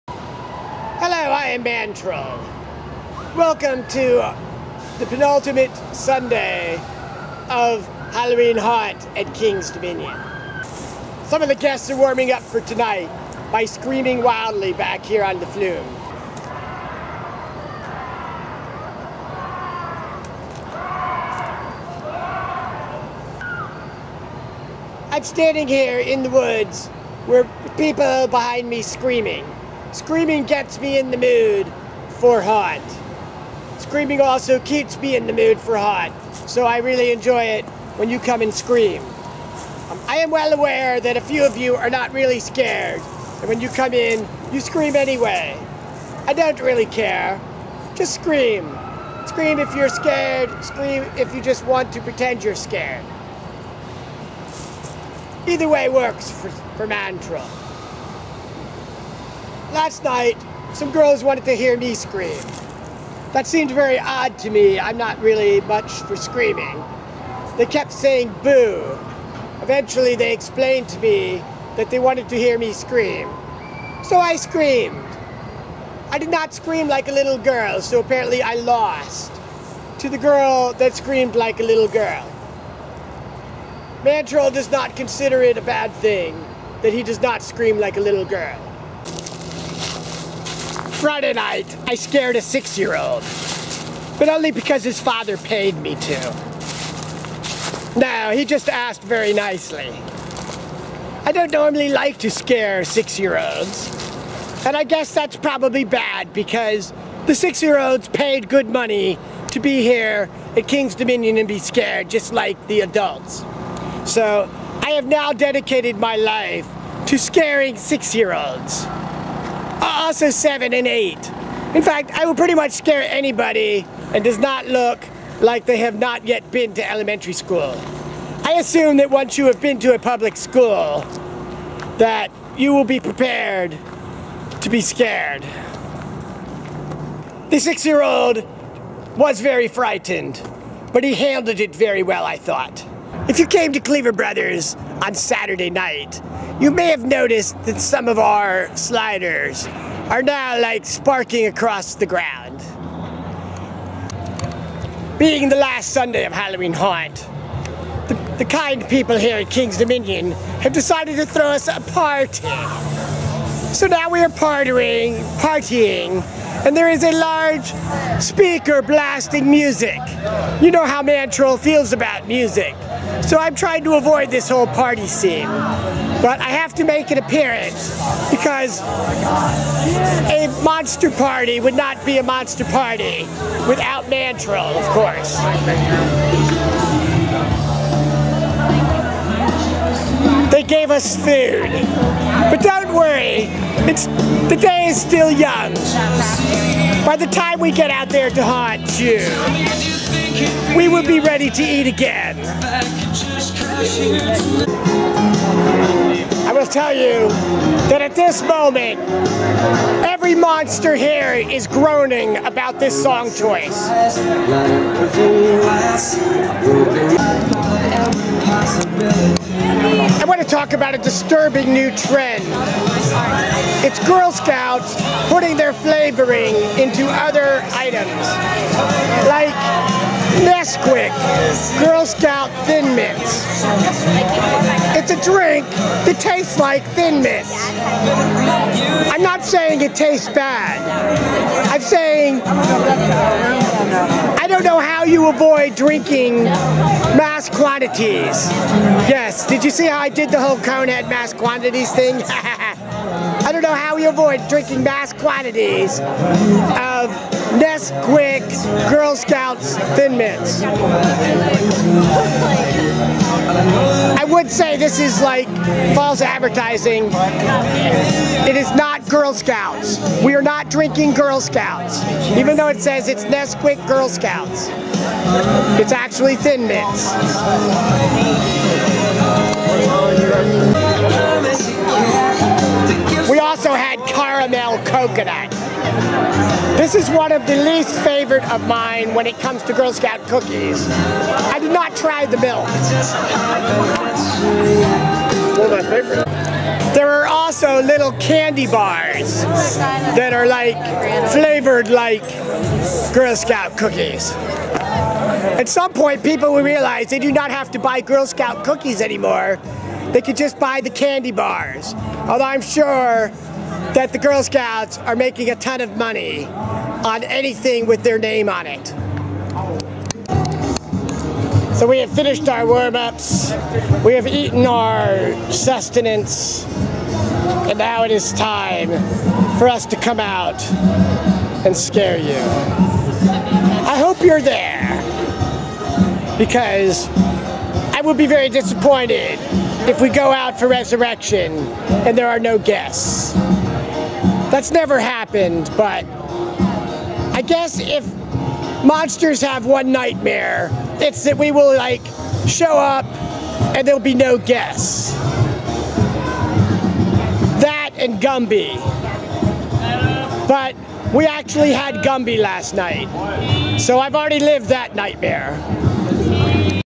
ManTroll: Vlog 3.15 - 10/26/2014 - Party
ManTroll discusses the Monster Bash, scaring little children, and other meaningless subjects.